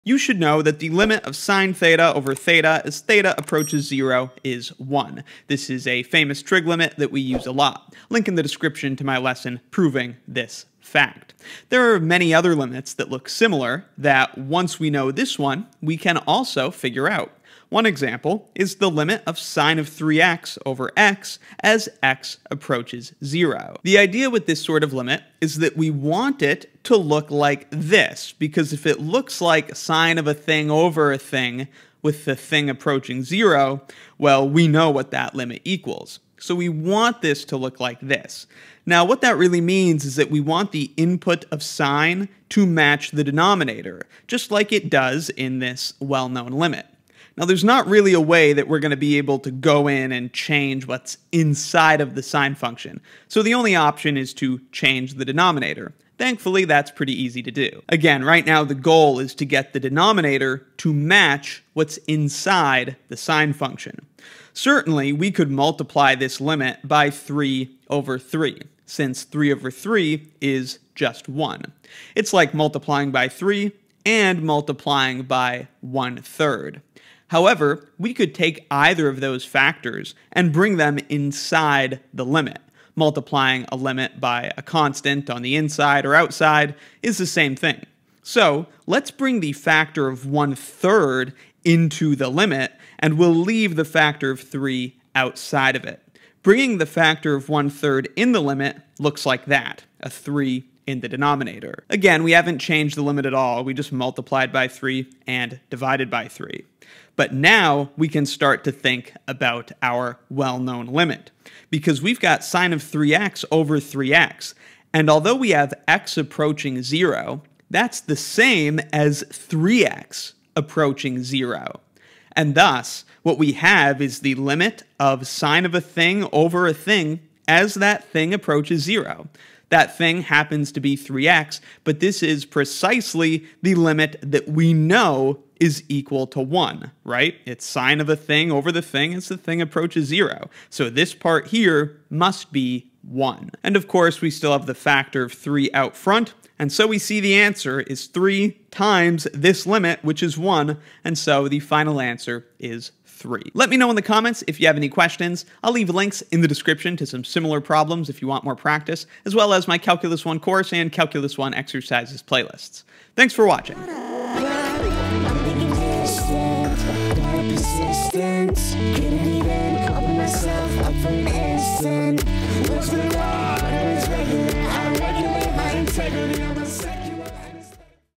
Limit of sin(3x)/x as x approaches 0 | Calculus 1 Exercises
We show the limit of sin(3x)/x as x goes to 0 is equal to 3. To evaluate this trigonometric limit, we need to remember the limit of sin(x)/x with x approaching 0, which is a fundamental trigonometric limit equal to 1! If we remember this, we'll be able to solve the problem in short order.